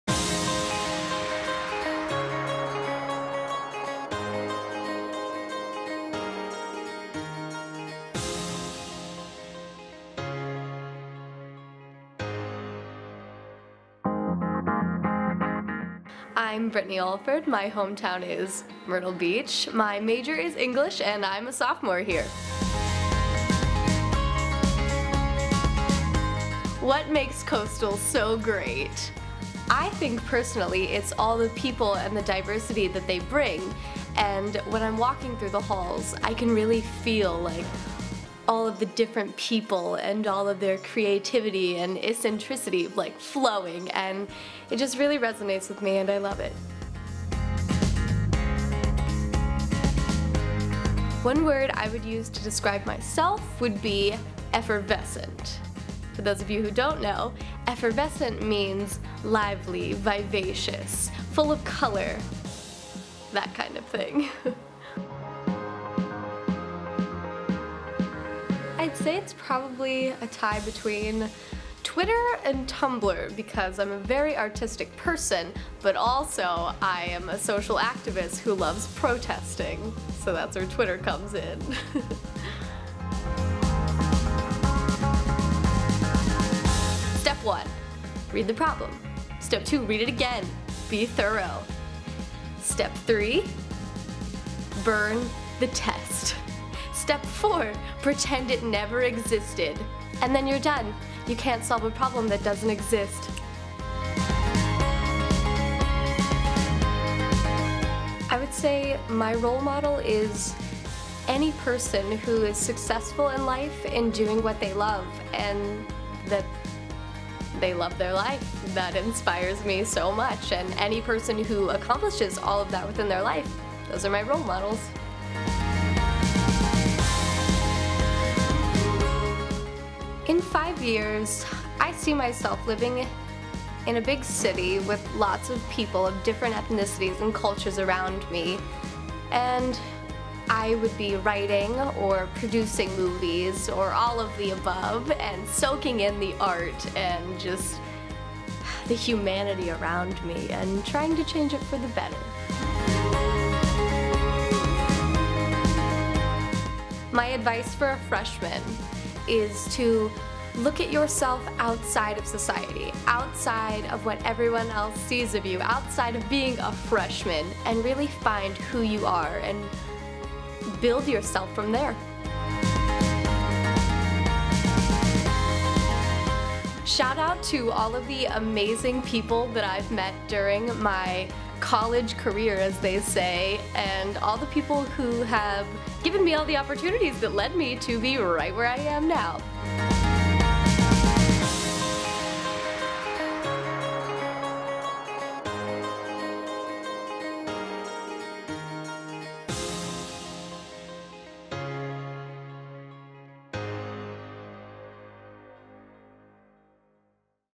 Brittany Alford
Brittany Alford is not boring. Meet her and feel the energy ooze out of her as it gravitates toward you. We aren’t kidding. She doesn’t describe herself as “effervescent” for nothing. This sophomore from Myrtle Beach is a passionate young woman who brings with her a one-of-a-kind personality.